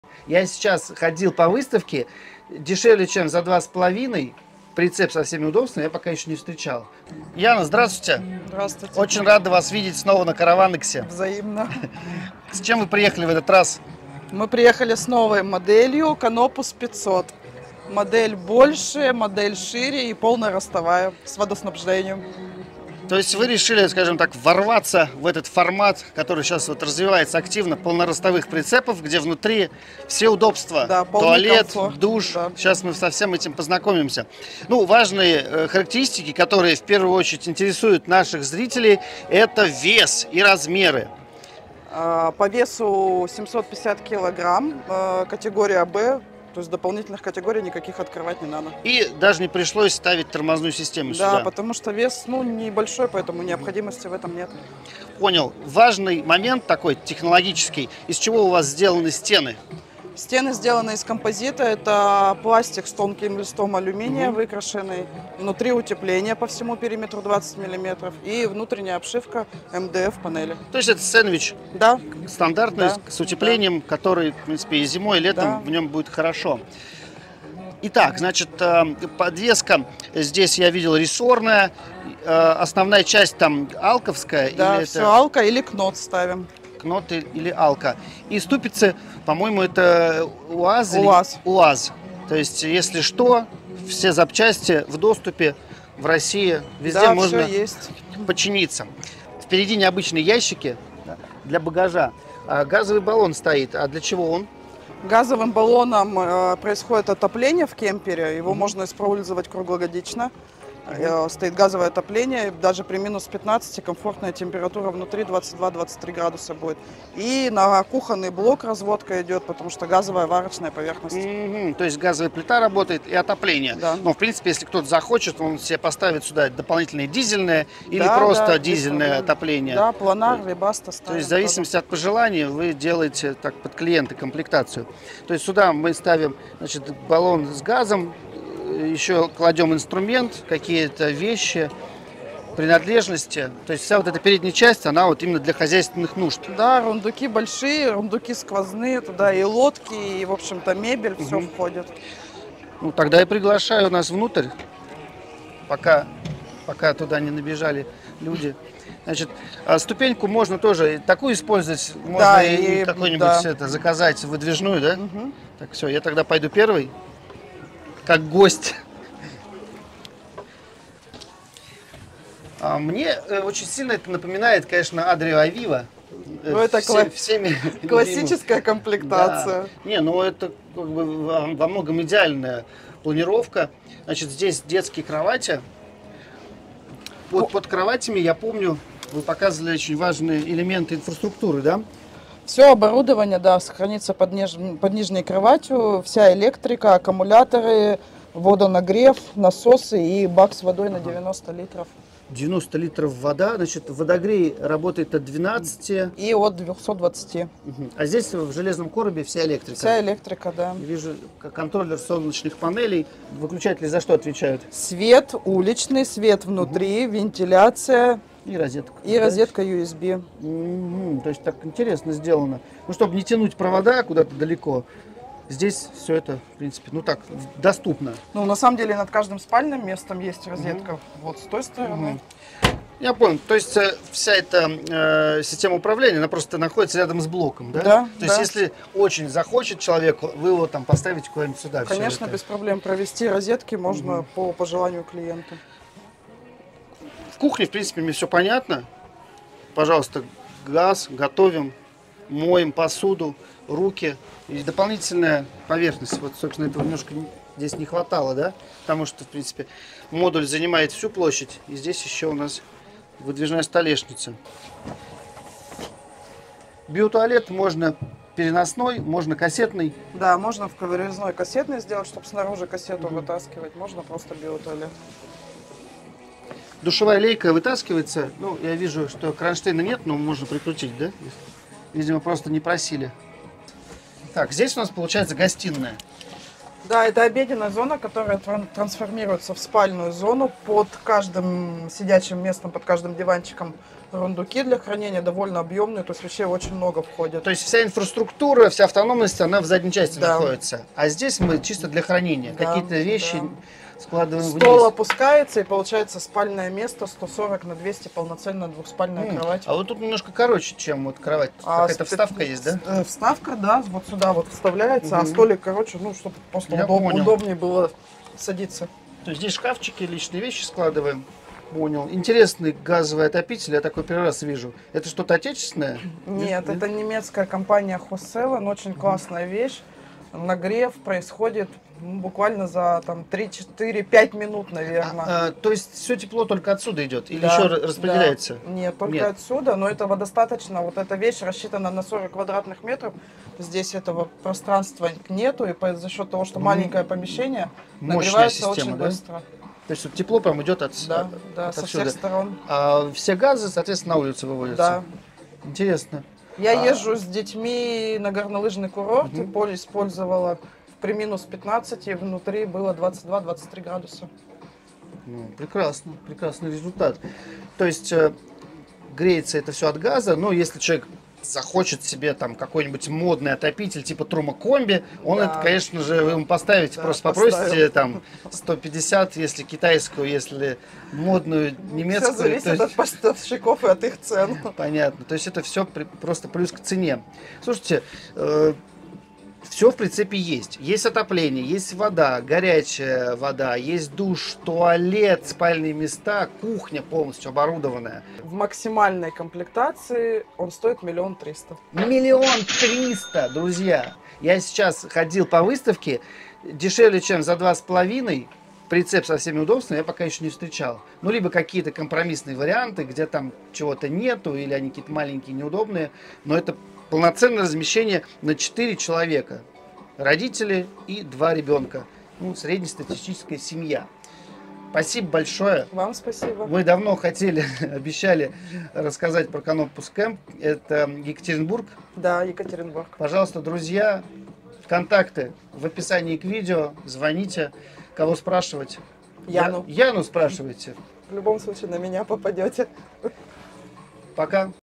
Дом на колесах Canopus Camp (г.Екатеринбург) с лучшей ценой на выставке
В обзоре караван, прицеп-дача российского производства Canopus Camp 500. Все удобства на борту для автономного проживания вчетвером.

Самое актуальное и интересное по теме автодомов:
Наш сайт: obzoravtodomov.ru